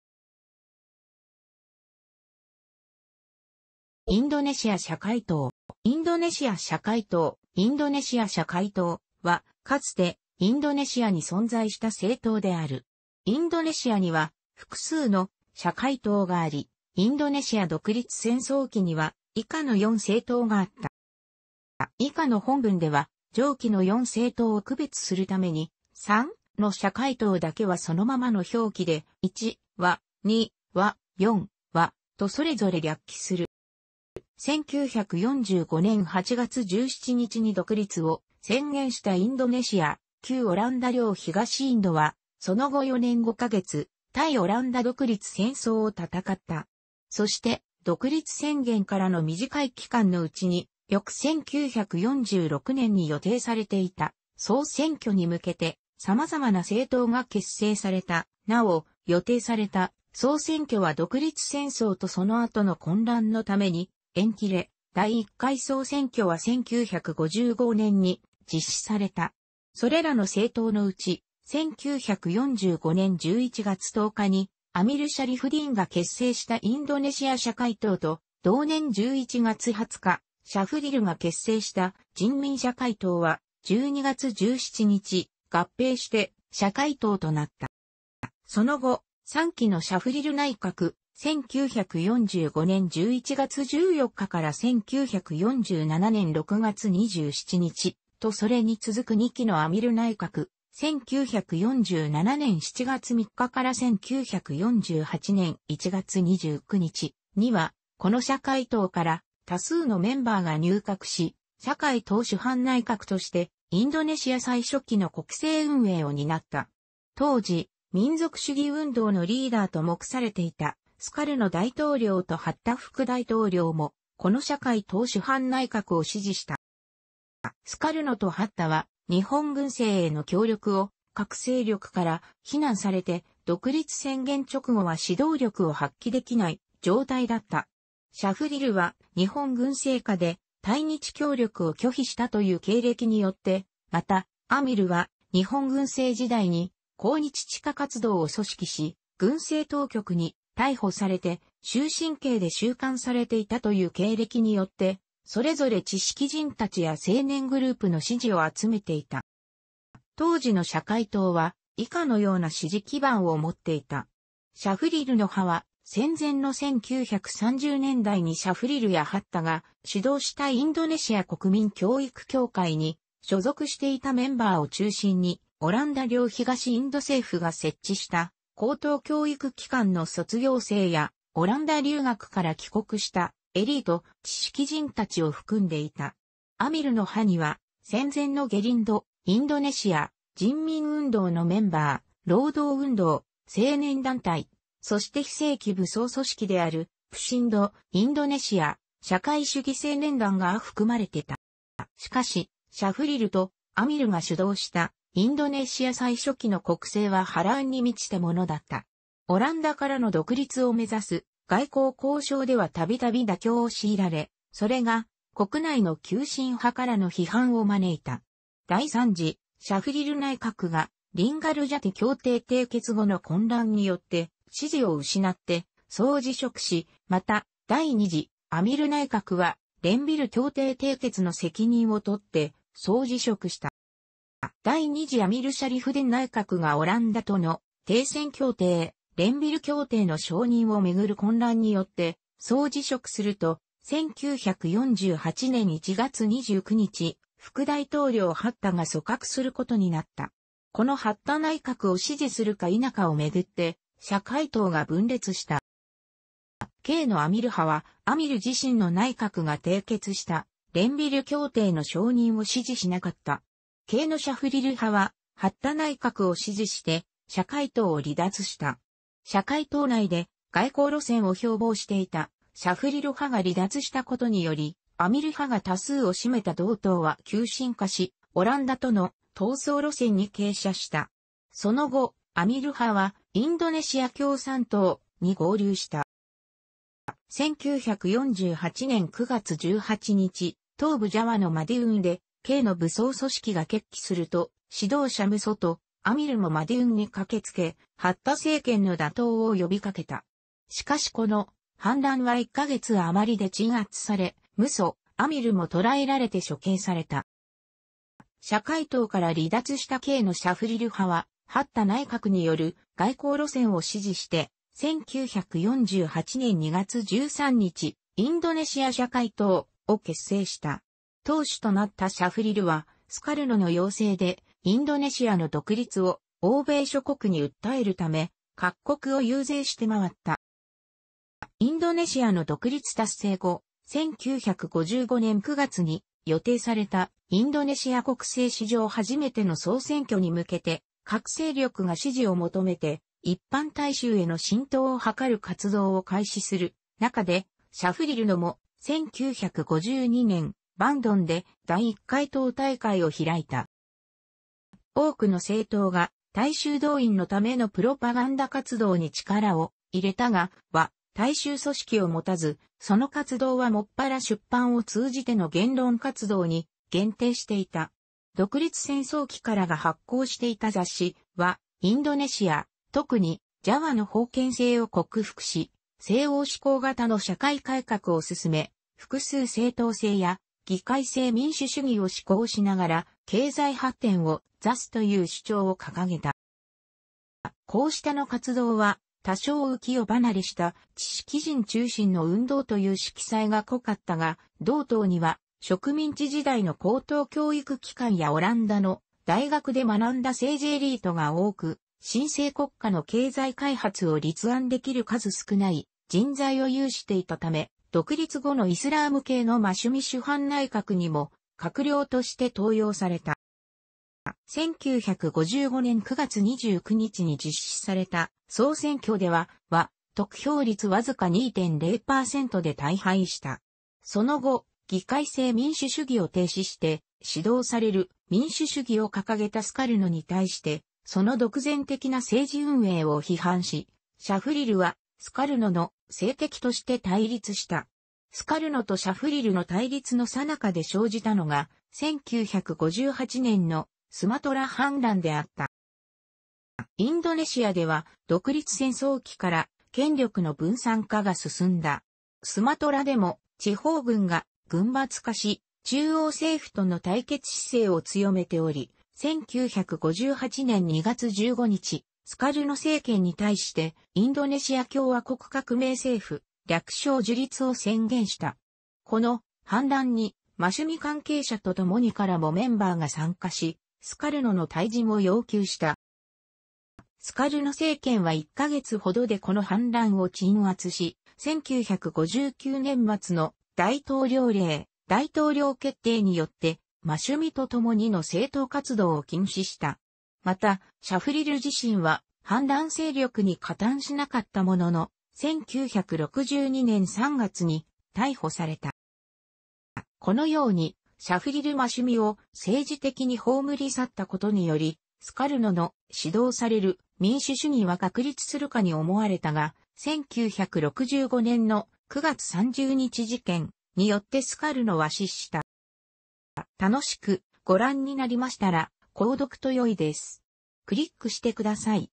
インドネシア社会党
インドネシア社会党（インドネシアしゃかいとう、) は、かつてインドネシアに存在した政党である。インドネシアには複数の「社会党」があり、インドネシア独立戦争期には以下の4政党があった。

以下の本文では上記の4政党を区別するために、（3）の社会党だけはそのままの表記で、（1）は、（2）は、（4）はとそれぞれ略記する。

1945年8月17日に独立を宣言したインドネシア（旧オランダ領東インド）は、その後4年5ヶ月、対オランダ独立戦争を戦った。そして、独立宣言からの短い期間のうちに、翌1946年に予定されていた総選挙に向けて、さまざまな政党が結成された（なお、予定された総選挙は独立戦争とその後の混乱のために延期され、第1回総選挙は1955年に実施された）。
それらの政党のうち、1945年11月10日にアミル・シャリフディンが結成したインドネシア社会党 と、同年11月20日、シャフリルが結成した人民社会党 は、12月17日、合併して社会党 となった。

その後、3期のシャフリル内閣（1945年11月14日-1947年6月27日）とそれに続く2期のアミル内閣（1947年7月3日-1948年1月29日）には、この社会党から多数のメンバーが入閣し、社会党首班内閣として、インドネシア最初期の国政運営を担った。当時、民族主義運動のリーダーと目されていたスカルノ大統領とハッタ副大統領も、この社会党首班内閣を支持した。

スカルノとハッタは、日本軍政への協力を各勢力から非難されて、独立宣言直後は指導力を発揮できない状態だった。シャフリルは日本軍政下で対日協力を拒否したという経歴によって、また、アミルは、日本軍政時代に抗日地下活動を組織し、軍政当局に逮捕されて終身刑で収監されていたという経歴によって、それぞれ知識人たちや青年グループの支持を集めていた。

当時の社会党は、以下のような支持基盤をもっていた。シャフリルの派は、戦前の1930年代にシャフリルやハッタが指導したインドネシア国民教育協会に所属していたメンバーを中心に、オランダ領東インド政府が設置した高等教育機関の卒業生や、オランダ留学から帰国したエリート、知識人たちを含んでいた。アミルの派には、戦前のゲリンド （、インドネシア人民運動）のメンバー、労働運動、青年団体、そして非正規武装組織であるプシンド （、インドネシア社会主義青年団）が含まれていた。

しかし、シャフリルとアミルが主導したインドネシア最初期の国政は波乱に満ちたものだった。オランダからの独立を目指す外交交渉ではたびたび妥協を強いられ、それが国内の急進派からの批判を招いた。第3次シャフリル内閣がリンガルジャティ協定締結後の混乱によって支持を失って総辞職し、また第2次アミル内閣は、レンヴィル協定締結の責任をとって総辞職した。

第2次アミル・シャリフディン内閣がオランダとの停戦協定（レンヴィル協定）の承認をめぐる混乱によって総辞職すると、1948年1月29日、副大統領ハッタが組閣することになった。このハッタ内閣を支持するか否かをめぐって、社会党が分裂した。

系のアミル派は、アミル自身の内閣が締結したレンヴィル協定の承認を支持しなかった。系のシャフリル派はハッタ内閣を支持して社会党を離脱した。社会党内で「外交路線」を標榜していたシャフリル派が離脱したことにより、アミル派が多数を占めた同党は急進化し、オランダとの「闘争路線」に傾斜した。その後、アミル派はインドネシア共産党（）に合流した。

1948年9月18日、東部ジャワのマディウンで、系の武装組織が決起すると、指導者ムソとアミルもマディウンに駆けつけ、ハッタ政権の打倒を呼びかけた。しかしこの「反乱」は1ヶ月あまりで鎮圧され、ムソ、アミルも捕らえられて処刑された。

社会党から離脱した系のシャフリル派は、ハッタ内閣による外交路線を支持して、1948年2月13日、インドネシア社会党（）を結成した。党首となったシャフリルは、スカルノの要請で、インドネシアの独立を欧米諸国に訴えるため、各国を遊説してまわった。

インドネシアの独立達成後、1955年9月に予定されたインドネシア国政史上初めての総選挙に向けて、各勢力が支持を求めて一般大衆への浸透をはかる活動を開始するなかで、シャフリルのも、1952年、バンドンで第1回党大会を開いた。

多くの政党が大衆動員のためのプロパガンダ活動に力を入れたが、は大衆組織を持たず、その活動はもっぱら出版を通じての言論活動に限定し...